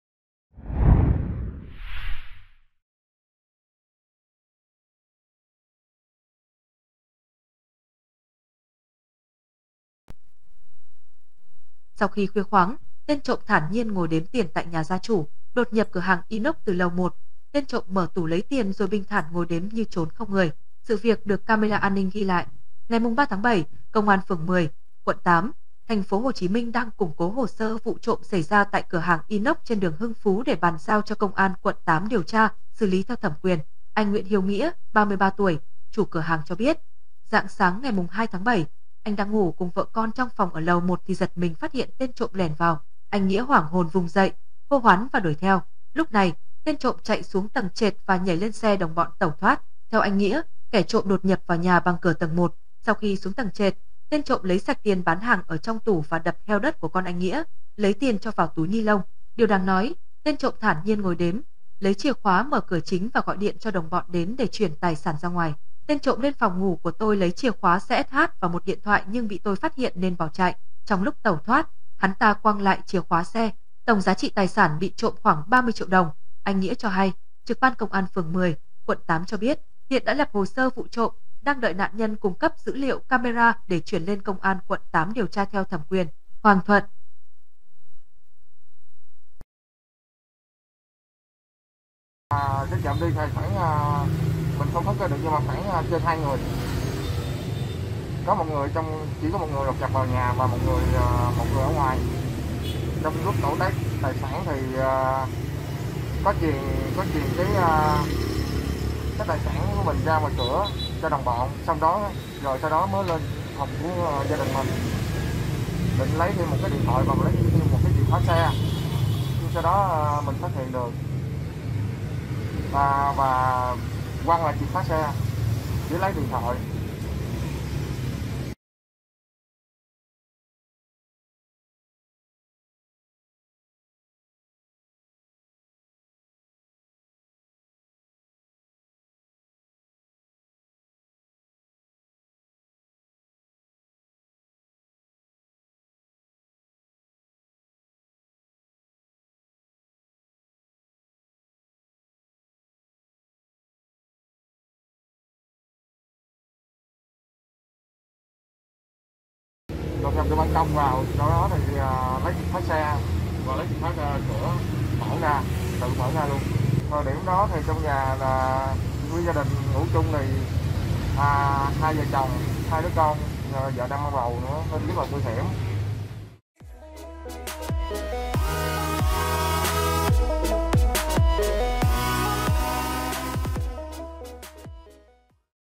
Sau khi khuya khoắng, tên trộm thản nhiên ngồi đếm tiền tại nhà gia chủ
Đột nhập cửa hàng inox từ lầu 1, tên trộm mở tủ lấy tiền rồi bình thản ngồi đếm như chốn không người. Sự việc được camera an ninh ghi lại.
Ngày 3/7, Công an phường 10 (Quận 8, TPHCM) đang củng cố hồ sơ vụ trộm xảy ra tại cửa hàng inox trên đường Hưng Phú để bàn giao cho Công an Quận 8 điều tra, xử lý theo thẩm quyền.
Anh Nguyễn Hiếu Nghĩa (33 tuổi, chủ cửa hàng) cho biết, rạng sáng ngày 2/7, anh đang ngủ cùng vợ con trong phòng ở lầu 1 thì giật mình phát hiện tên trộm lẻn vào. Anh Nghĩa hoảng hồn vùng dậy, hô hoán và đuổi theo.

Lúc này, tên trộm chạy xuống tầng trệt và nhảy lên xe đồng bọn tẩu thoát. Theo anh Nghĩa, kẻ trộm đột nhập vào nhà bằng cửa tầng 1.

Sau khi xuống tầng trệt, tên trộm lấy sạch tiền bán hàng ở trong tủ và đập heo đất của con anh Nghĩa, lấy tiền cho vào túi nylon. Điều đáng nói, tên trộm thản nhiên ngồi đếm, lấy chìa khóa mở cửa chính và gọi điện cho đồng bọn đến để chuyển tài sản ra ngoài.
"Tên trộm lên phòng ngủ của tôi lấy chìa khóa xe SH và một điện thoại nhưng bị tôi phát hiện nên bỏ chạy. Trong lúc tẩu thoát, hắn ta quăng lại chìa khóa xe. Tổng giá trị tài sản bị trộm khoảng 30 triệu đồng", anh Nghĩa cho hay.

Trực ban Công an Phường 10 - Quận 8 cho biết: Hiện đã lập hồ sơ vụ trộm, đang đợi nạn nhân cung cấp dữ liệu camera để chuyển lên Công an quận 8 điều tra theo thẩm quyền.